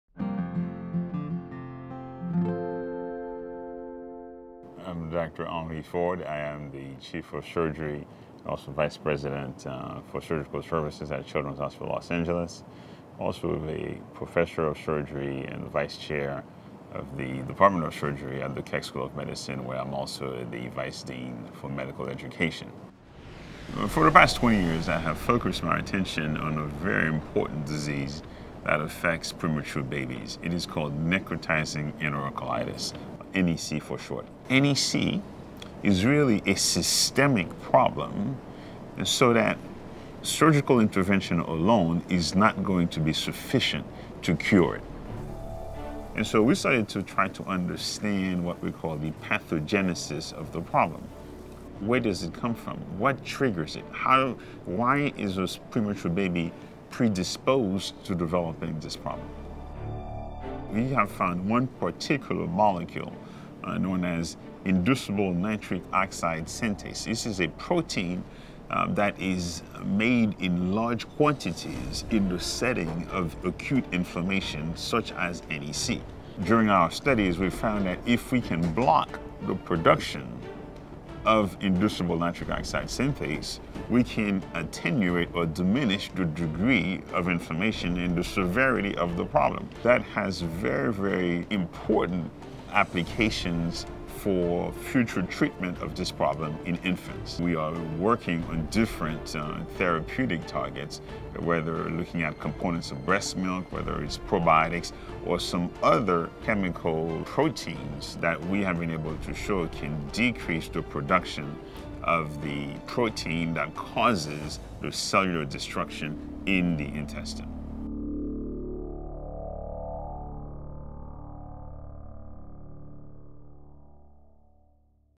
Henri R. Ford, MD, MHA, Vice President and Chief of Surgery
Necrotizing enterocolitis (NEC) is a severe and often life-threatening intestinal inflammation in pre-term infants. Using models of NEC, we are trying to understand how colonization with potentially harmful bacteria leads to inflammation, damage to the intestine and tissue necrosis. Our findings will lead to better diagnostics, prophylactics and treatments for NEC.For more information about Dr. Ford's research, visit CHLA.org/FordLab.